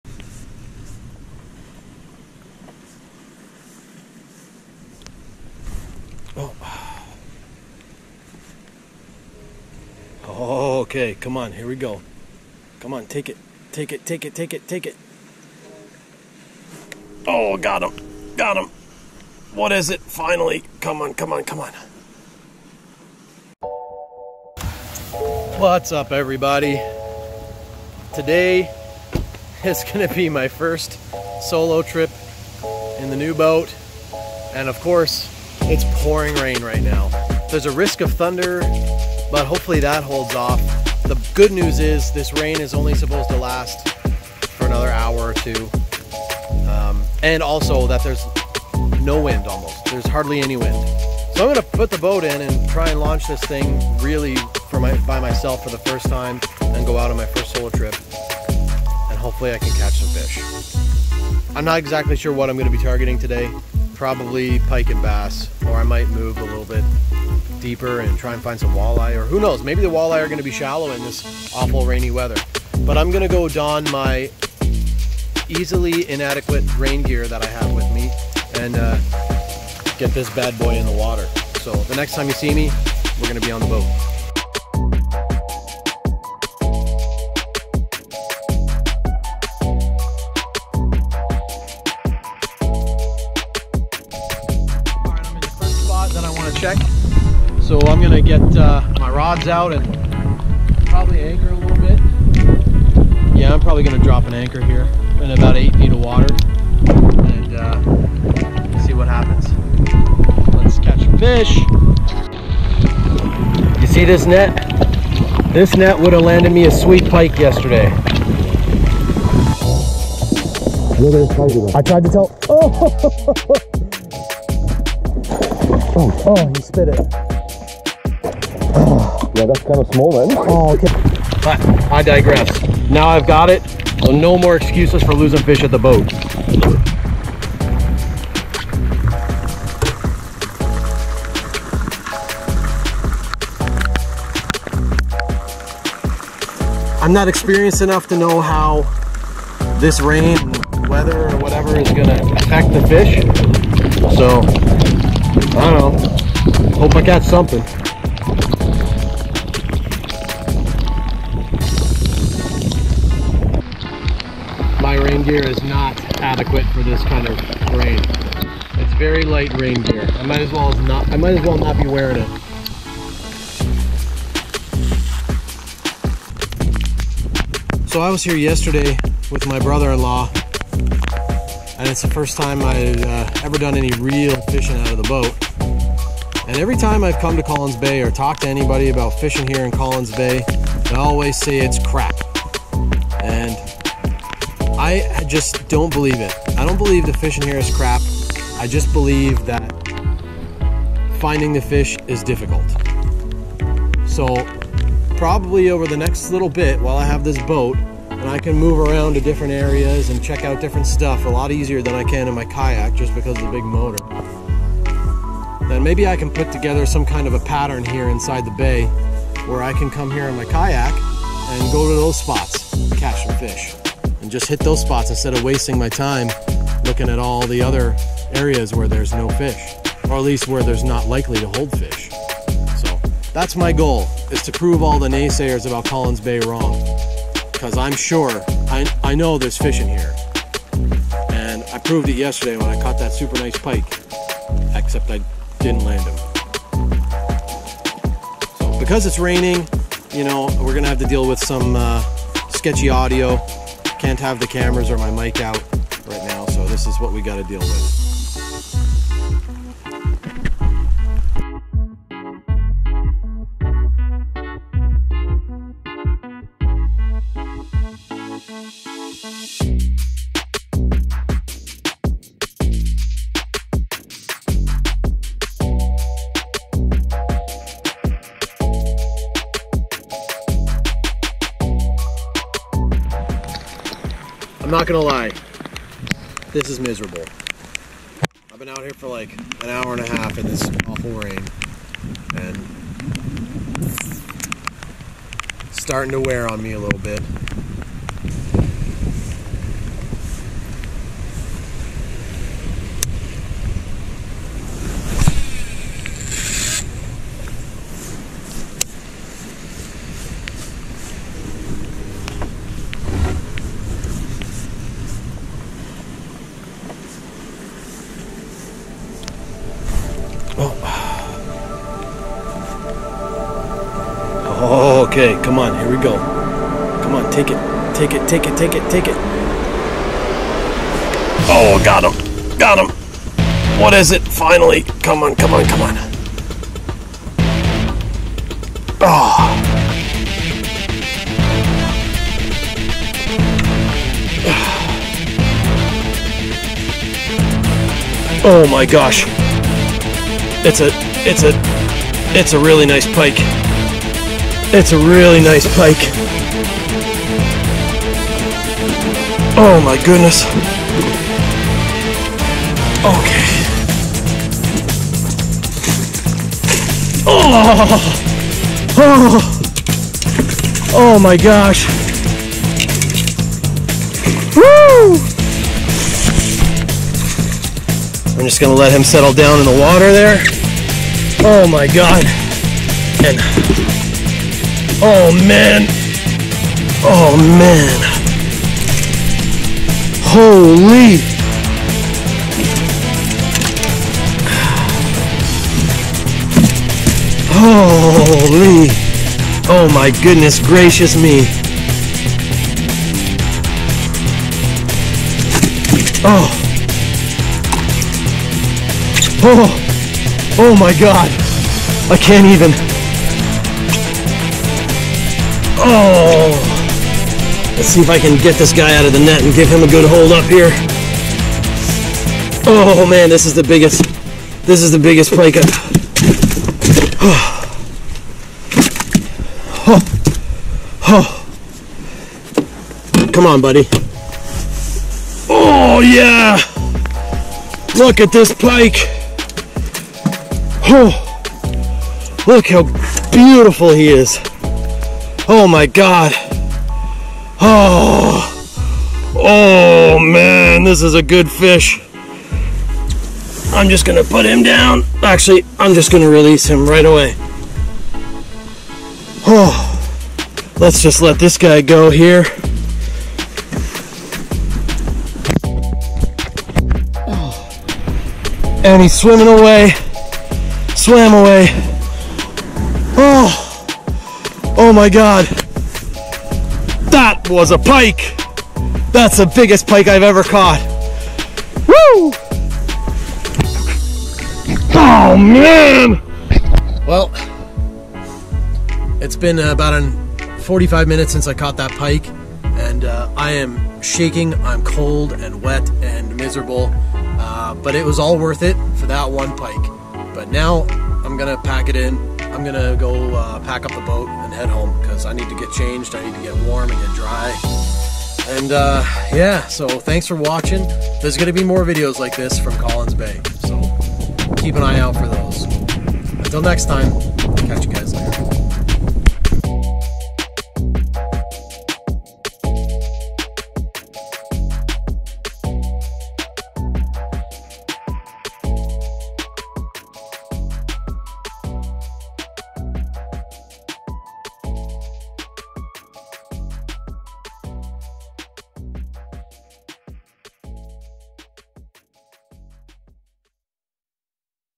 PB Northern Pike - First fish in the new boat! (Colins Bay)
I caught my personal best Northern Pike, on my first solo trip in the new boat, and he was the first big fish to slime the deck!

The forecast said that the rain was supposed to stop around 10-11 o'clock and of course, it didn't. It rained the entire time I was out! At least I was ready for a big fish this time. The day before I lost a really nice pike at the boat because my net was too small, but after I parked the boat at home I went straight to buy a new net, and that net landed me this gorgeous Pike.

After fishing for 2+ hours without catching anything, I switched to a pink Northland Whistler Jig and put on a chartruse swimbait I made that I had left over from a previous order and that did the trick in the green tinted water of Colin's Bay!